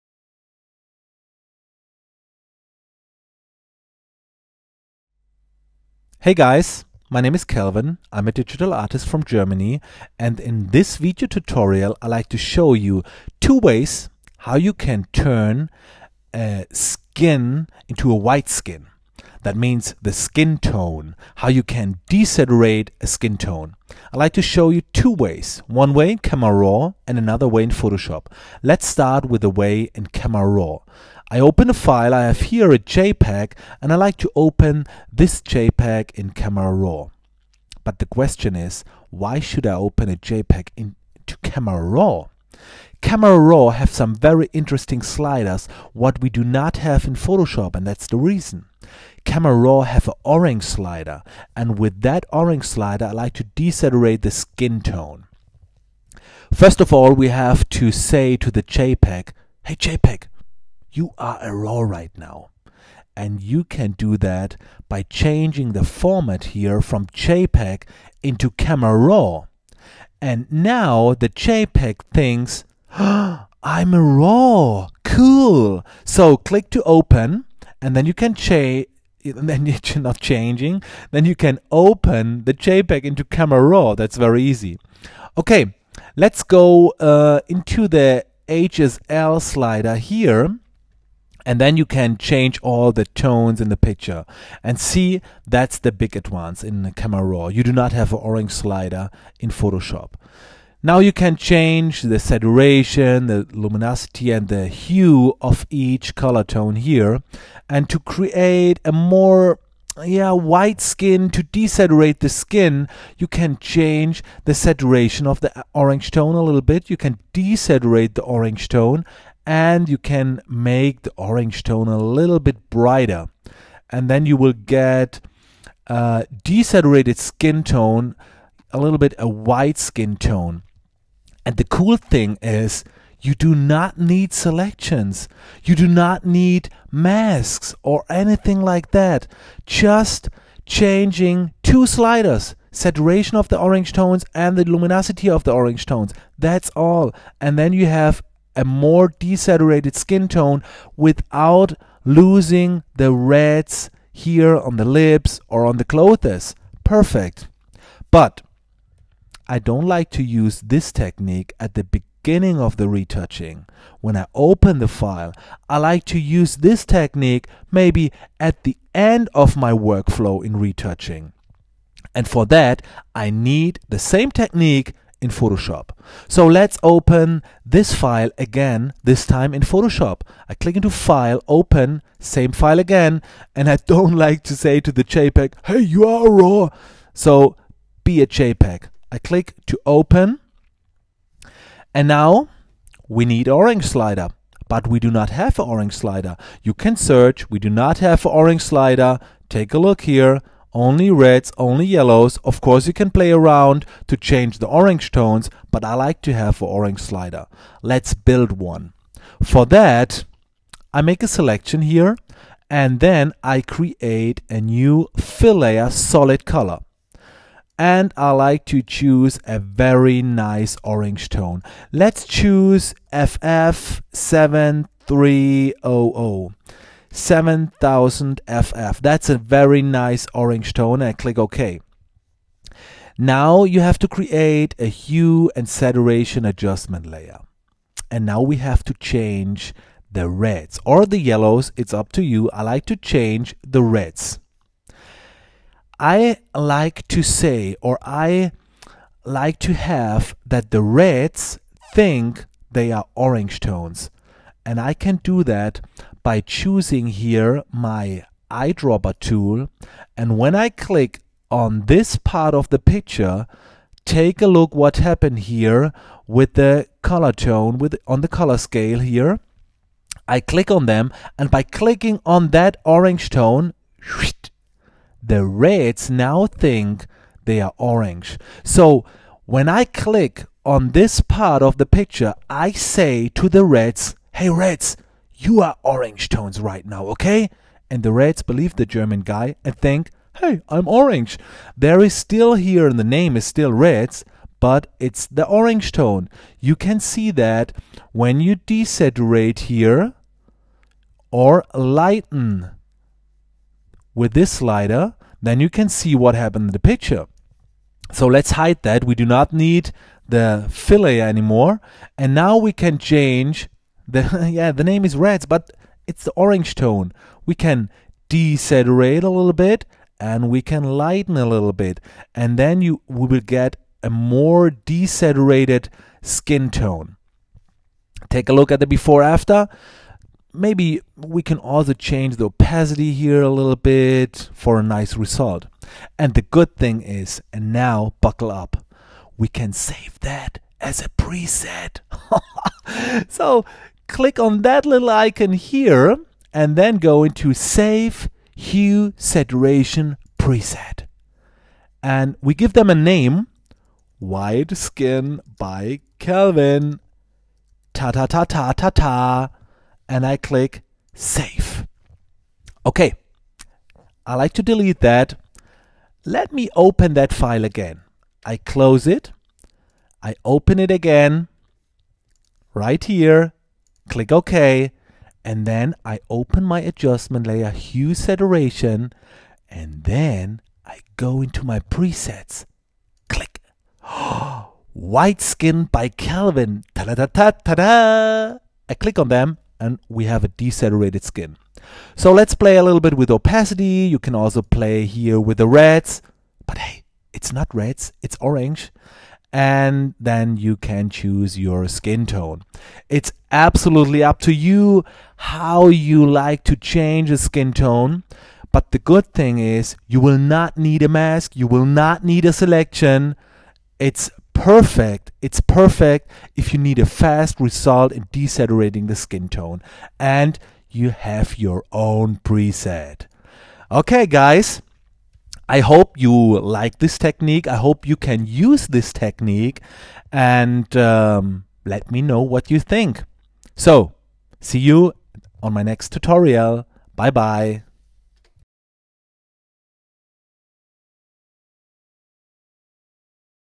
Skin desaturation.mp4
In this tutorial i like to show you a technique to desaturate a skin